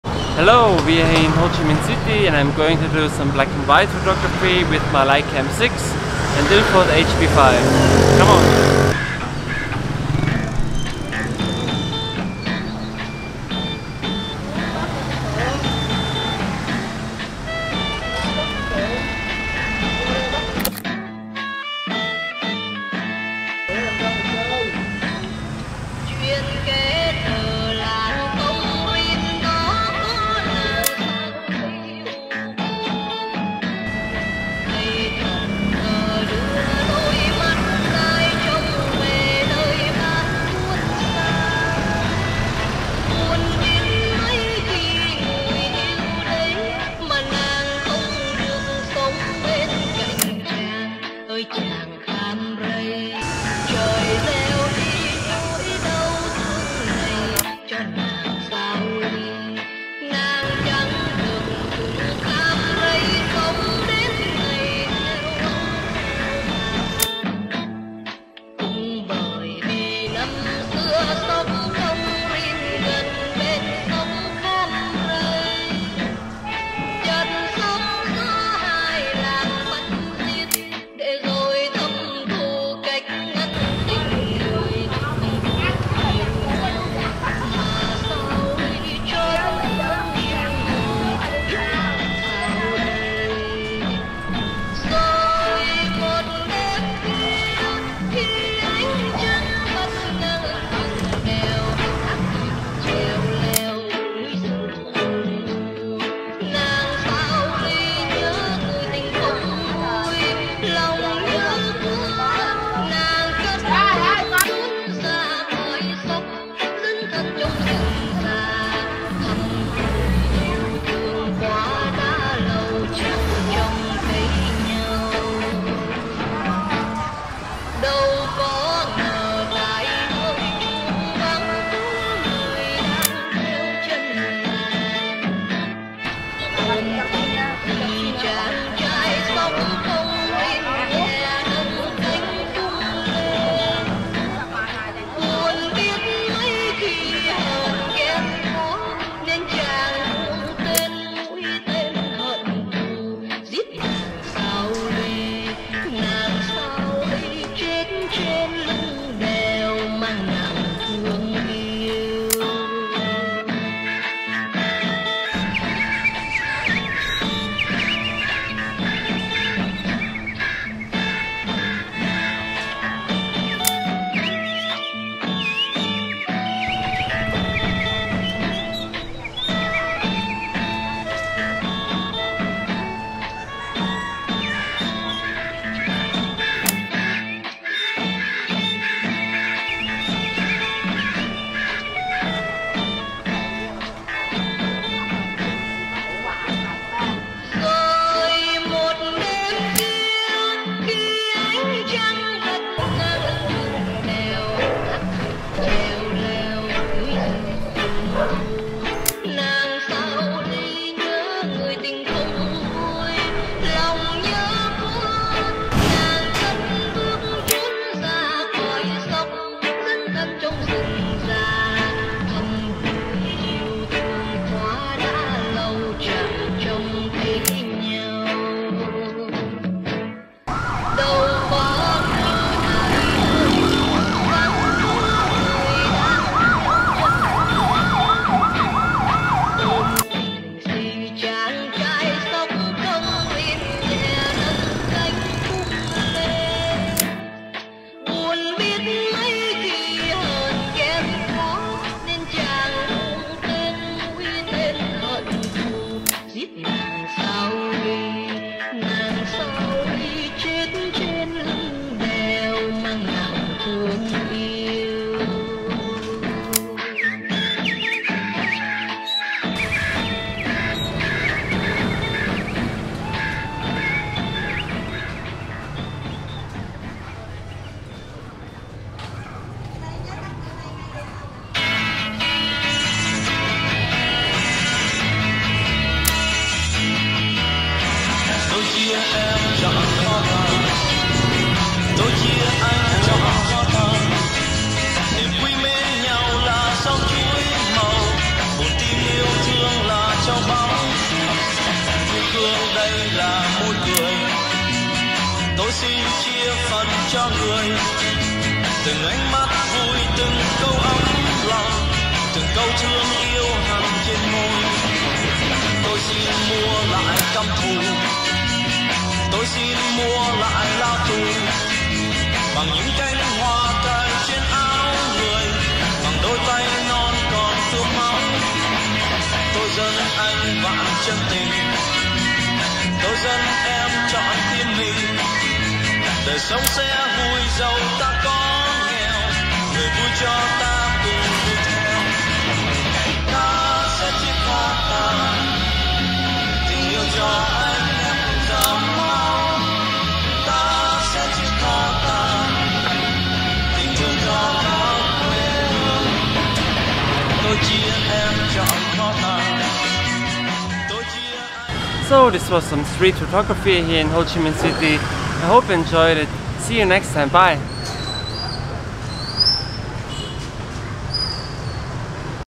35mm Street Photography in Vietnam | Saigon
Here is a new episode of analog 35mm street photography in Vietnam, Saigon/Ho Chi Minh City.

Camera: Leica M6
Lens: Leica Summicron 35mm f2
Film: Ilford HP5